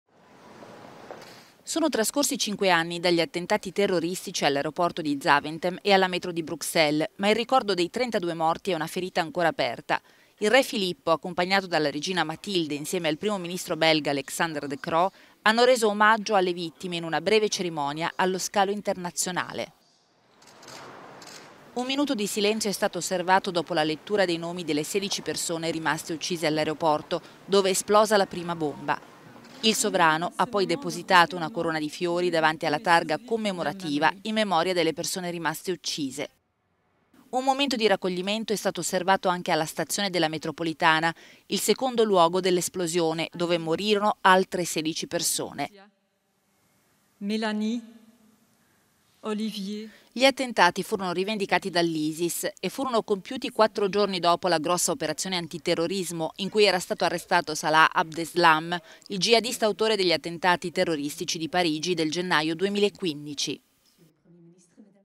Bruxelles si ferma in ricordo delle vittime degli attentati del 2016
Un minuto di silenzio e una cerimonia sobria per ricordare gli attentati di 5 anni fa all'aeroporto e alla stazione della metro di Bruxelles